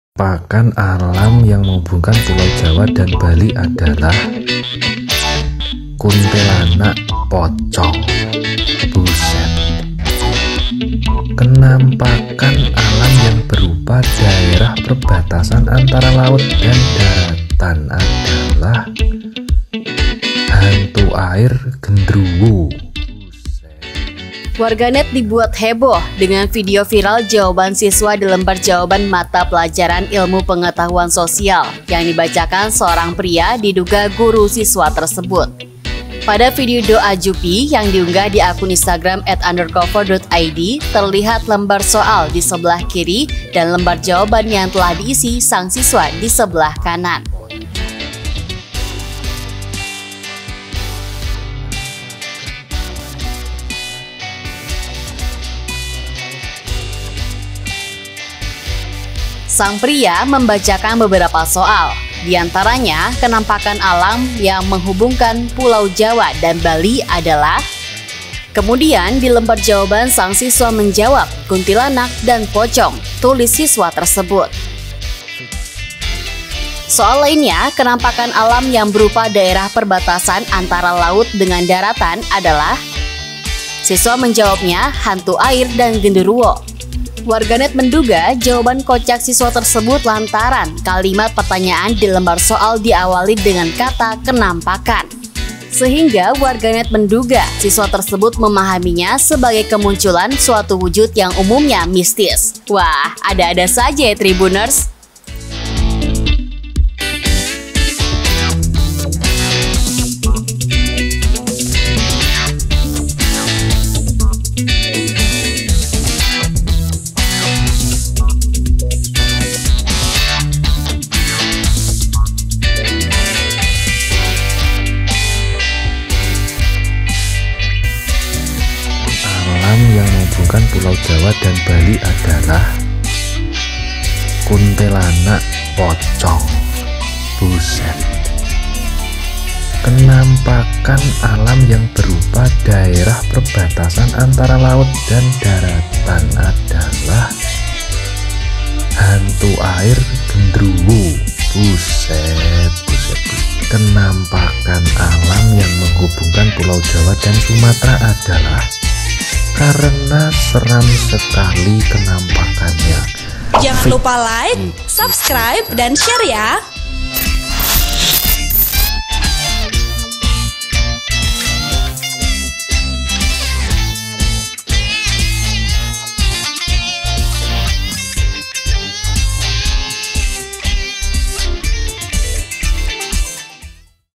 ®️ 🔴 Viral Jawaban Kocak Siswa pada Soal IPS
Warga net dibuat heboh, dengan video viral jawaban siswa di lembar jawaban mata pelajaran Ilmu Pengetahuan Sosial, yang dibacakan seorang pria diduga guru siswa tersebut.

Pada video doa_juppi yang diunggah akun Instagram underc0ver.id
Terlihat lembar soal di sebelah kiri dan lembar jawaban yang telah diisi sang siswa di sebelah kanan.

Sang pria, membacakan beberapa soal. Di antaranya “Kenampakan alam yang menghubungkan pulau Jawa dan Bali adalah…”.
Kemudian di lembar jawaban, sang siswa menjawab “Kuntilanak, Pocong,” tulis siswa tersebut.

Soal lainnya, “Kenampakan alam yang berupa daerah perbatasan antara laut dengan daratan adalah …”. Siswa menjawabnya “ Hantu air, gendruo”.
Warga net menduga, jawaban kocak siswa tersebut, lantaran kalimat pertanyaan di lembar soal di awali dengan kata “Kenampakan”.

Sehingga warga net menduga, siswa tersebut memahaminya sebagai kemunculan suatu wujud yang umumnya mistis.
Wah, ada-ada saja ya tribuners